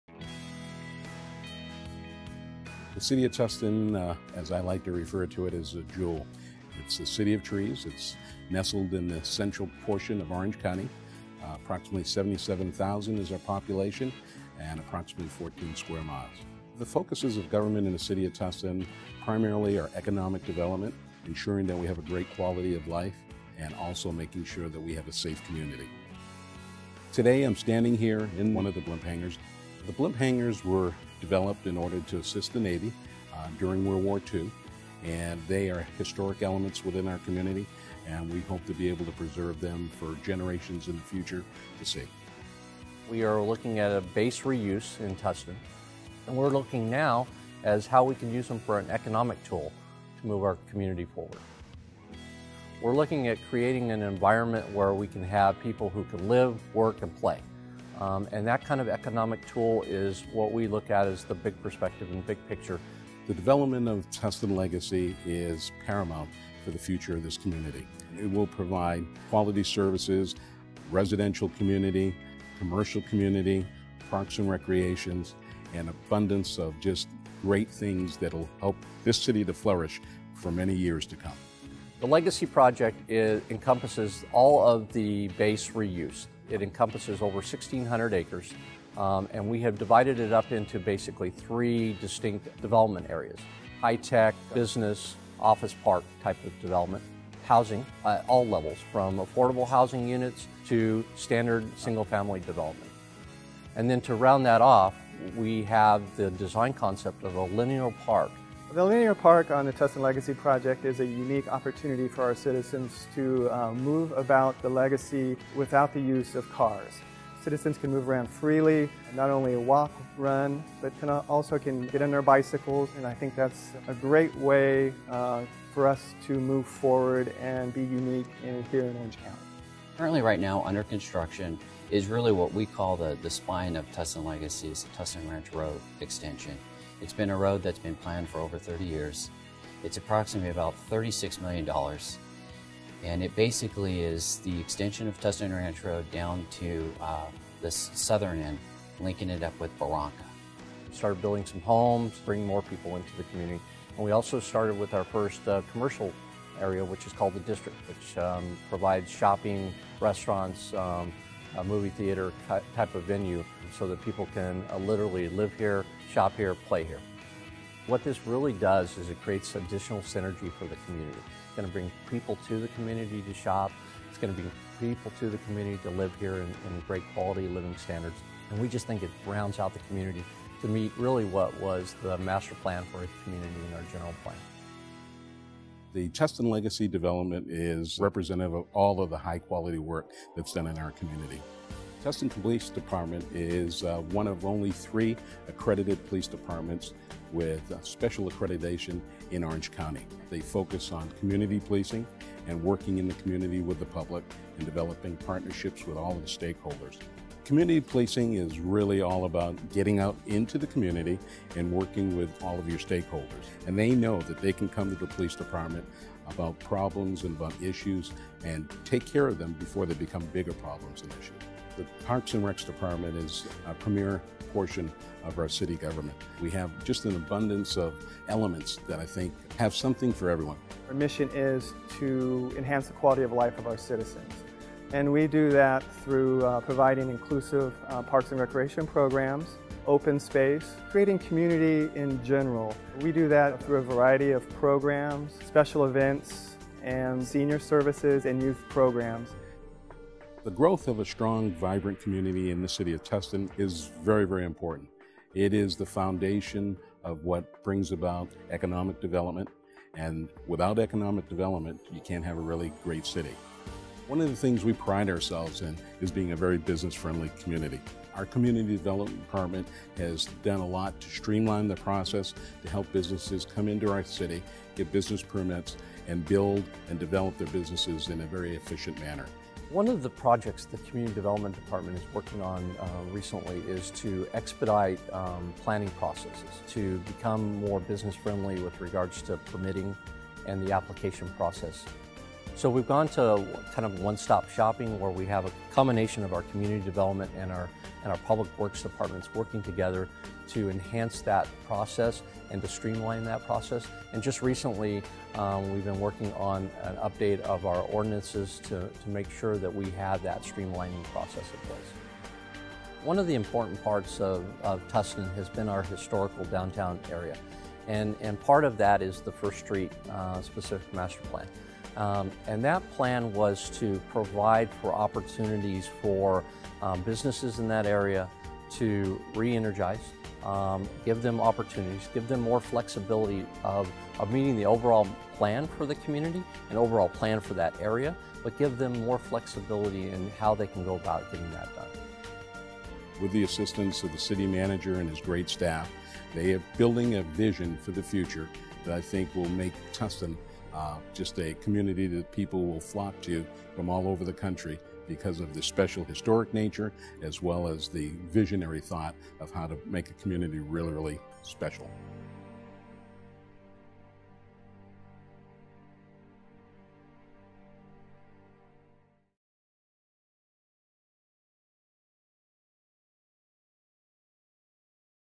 City of Tustin, California - Legacy and Future
Tustin, CA is home to two huge World War Two era blimp hangars. This legacy forms the centrepiece to Tustin's Legacy Project which sees growth and regeneration for citizens of Tustin at the heart of Orange County. With a new park, shopping facilities, housing and business opportunities, Tustin seeks to continue to grow well into the future.